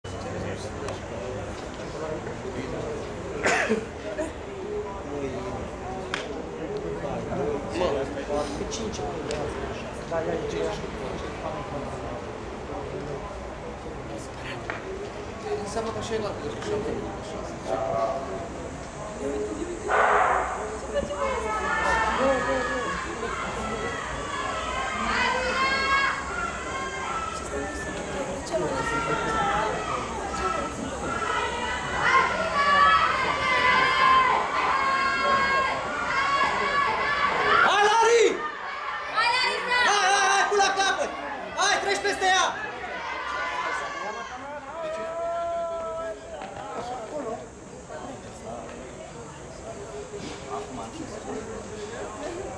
F 200 m Heat 2 CN J2 Finala 14 06 2015
2015 - June 14 - Track & Field - Romanian Under 18 National Championships - Final (Day 2)